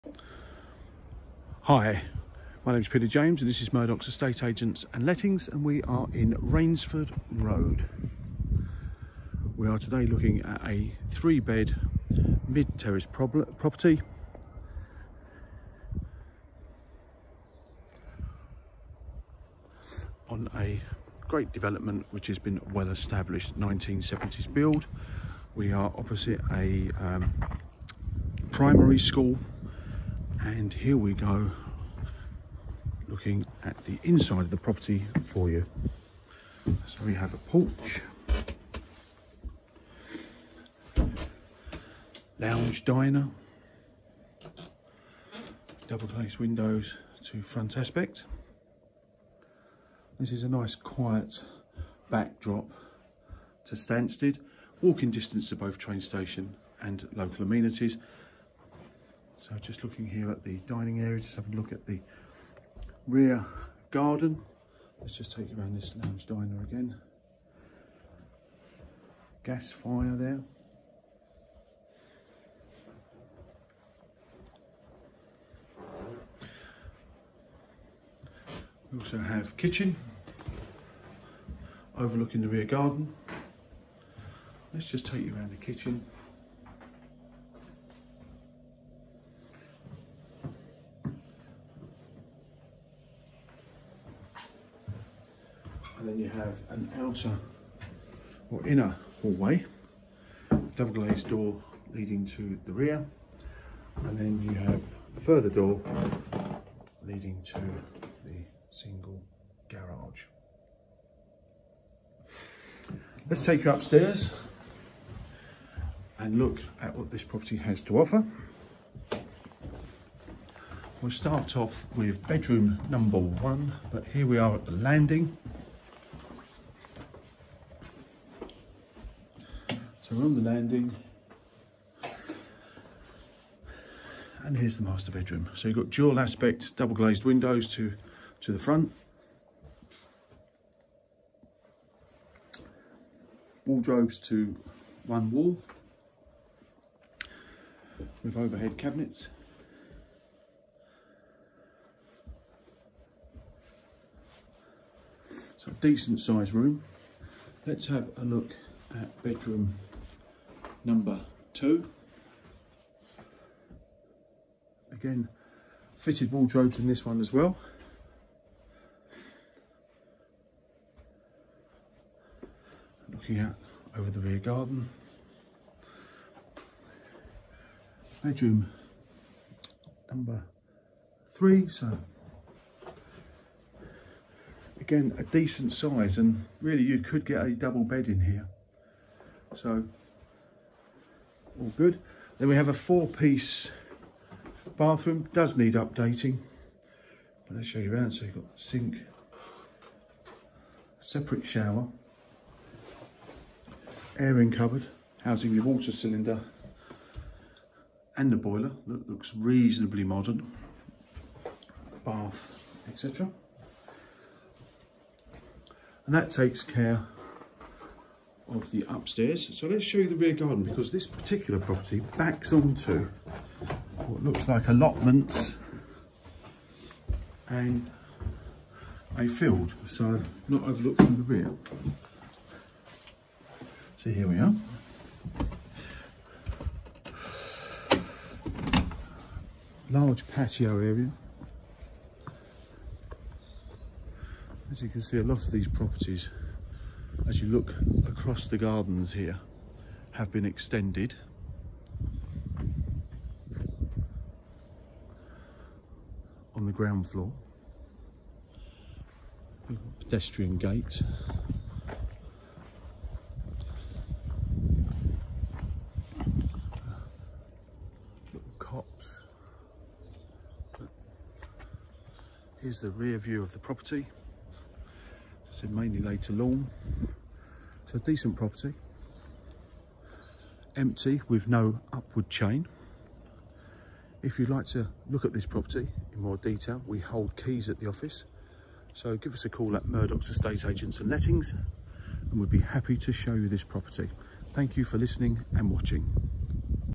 Rainsford Road, Stansted Mountfitchet.
This 3 bedroom terraced property is offered with NO ONWARD CHAIN. The accommodation in brief comprises of open plan living room and dining area, fitted kitchen, family bathroom, garage and off road parking, garden. REQUIRING MODERNISATION.